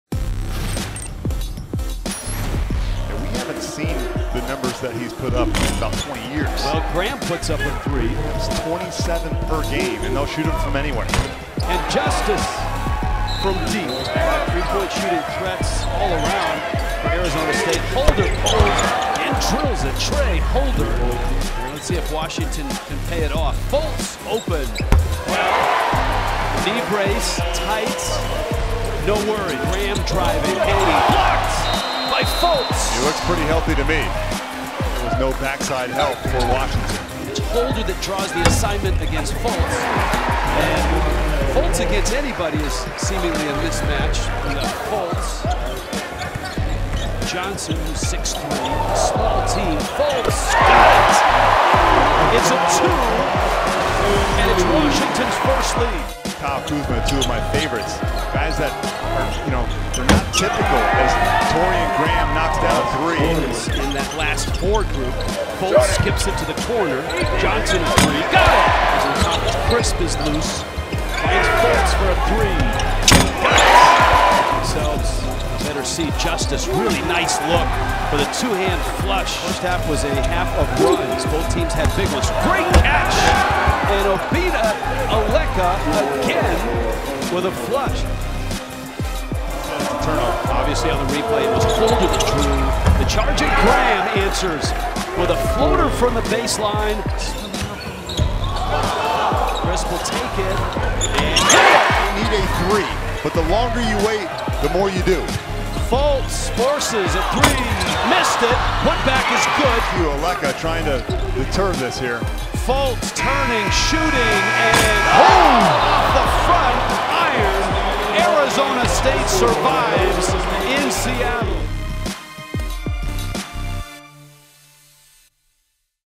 Highlights: Arizona State men's basketball survives Washington in thriller
Highlights from Arizona State's 83-81 win over Washington on Thursday night in Seattle.